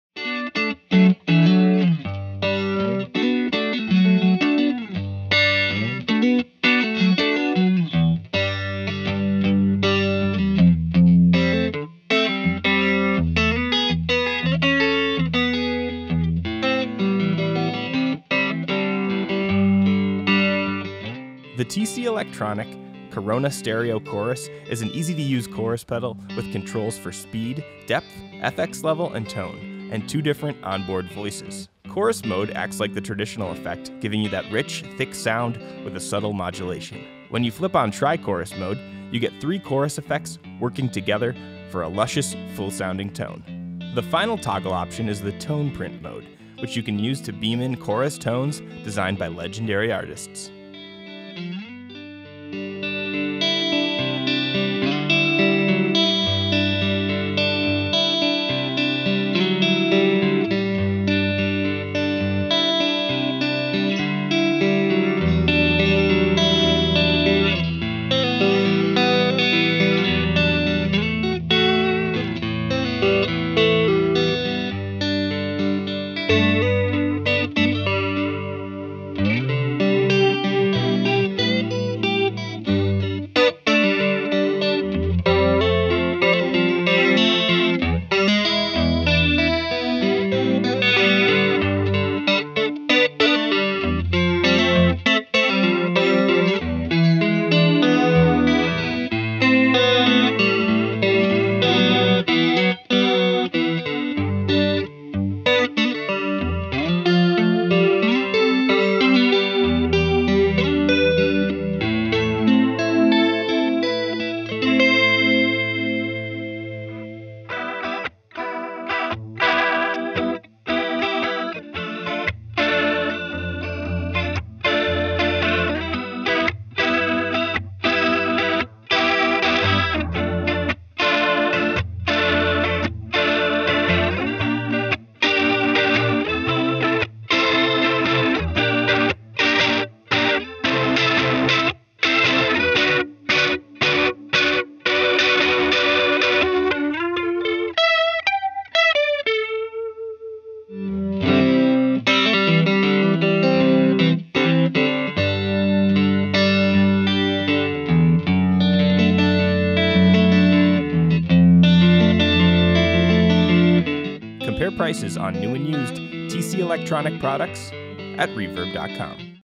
TC Electronic Corona Stereo Chorus | Reverb Demo Video
The TC Electronic Corona Stereo Chorus is an easy to use Chorus pedal with controls for Speed, Depth, FX level and Tone, and two different on-board voices. Chorus mode acts like the traditional effect, giving you that rich thick sound with subtle modulation. When you flip on Tri-Chorus mode, you get three chorus effects working together for a luscious, full sounding tone. The final toggle option is the ‘Tone Print’ mode, which you can use to beam chorus tones designed by legendary artists to your pedal.

Compare prices on new and used Corona Stereo Chorus pedals